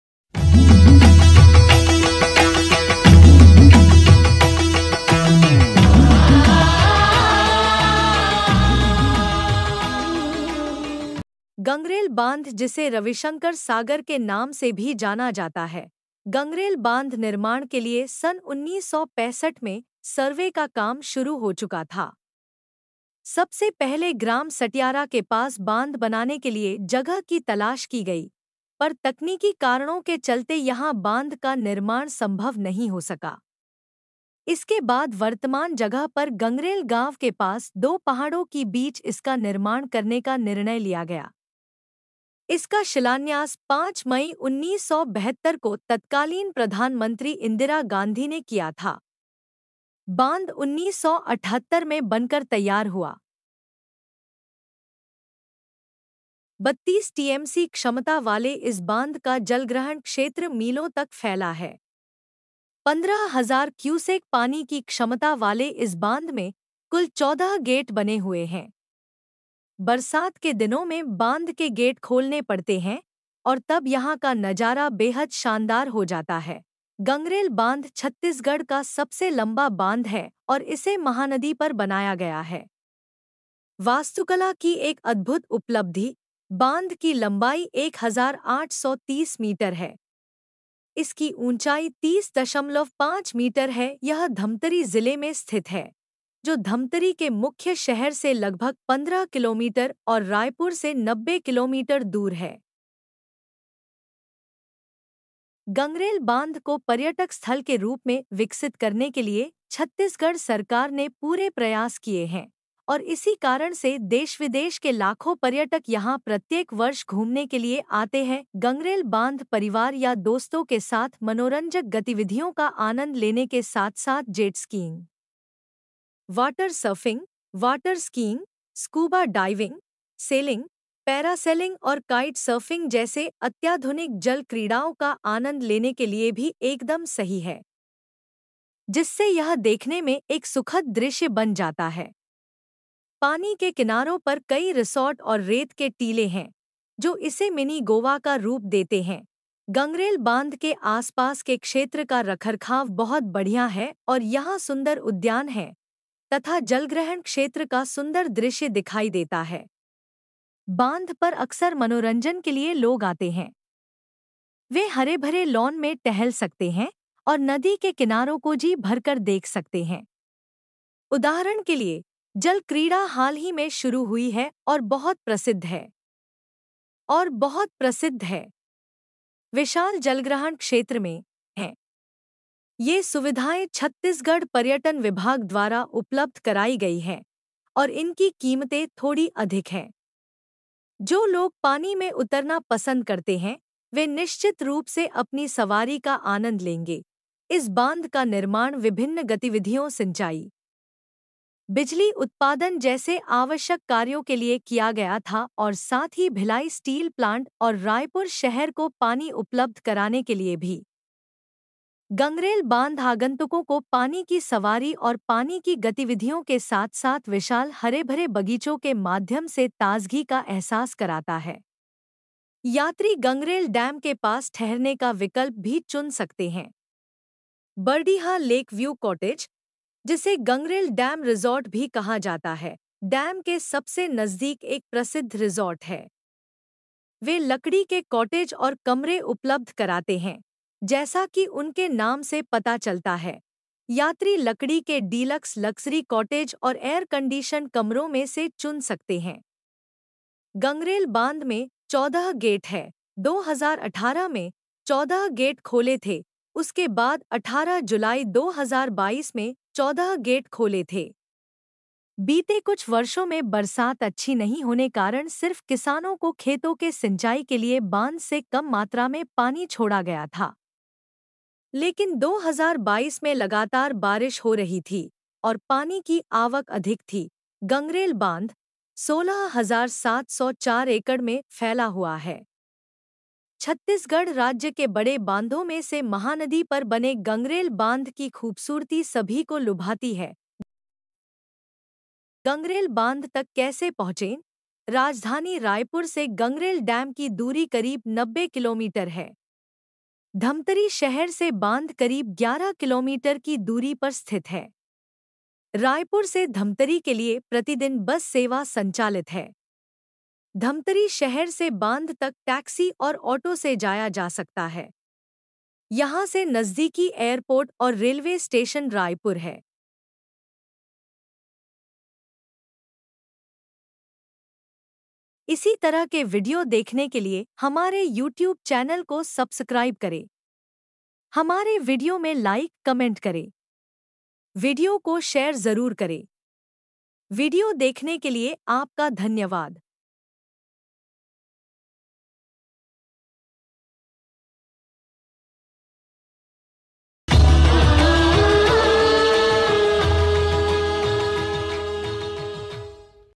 गंगरेल डैम धमतरी || छत्तीसगढ़ का सबसे लंबा बांध || 16 हजार 704 एकड़ में फैला |जलग्रहण का सुंदर दृश्य
GangrelDamIndia#[StateName]Tourism
#[DistrictName]Tourism
#[CityNearDam]Tourism#DamTourism
#[DistrictName]Tourism
#[CityNearBandh]Tourism